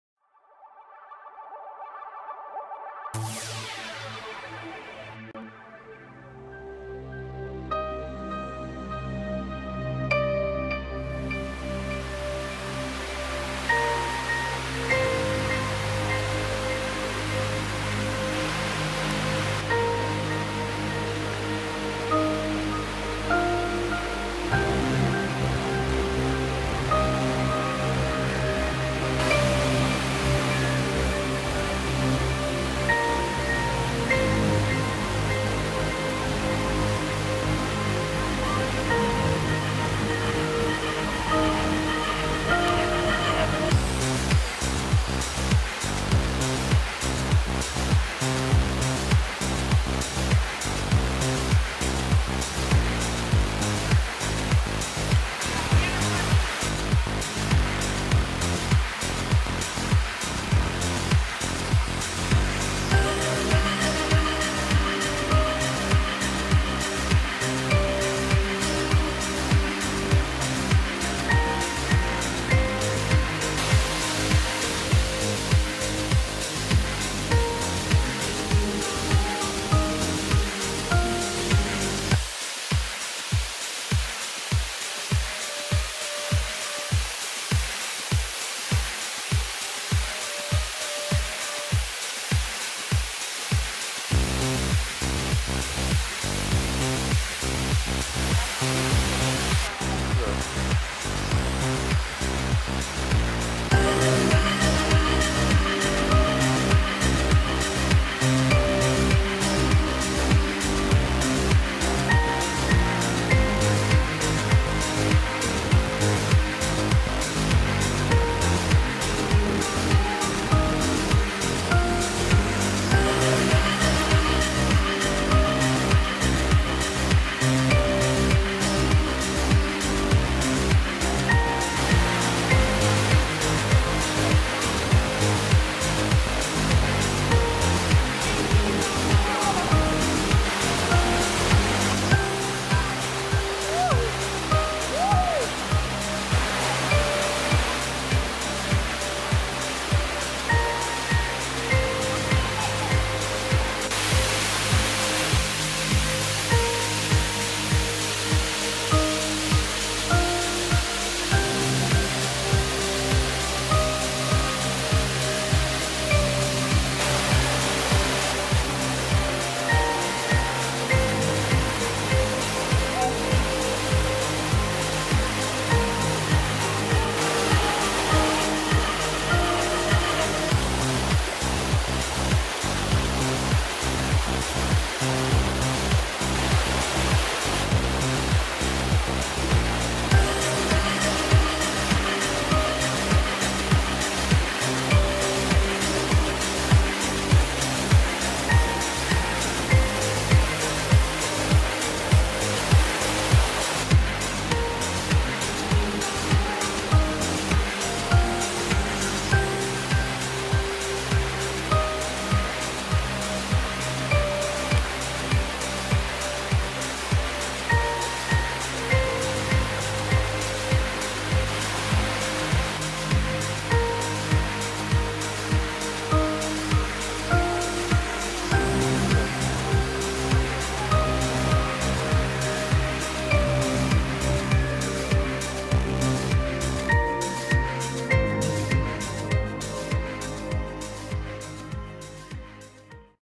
Lillafüred vízesései
A nagy hőségben,üdítő képsorok Lillafüredről!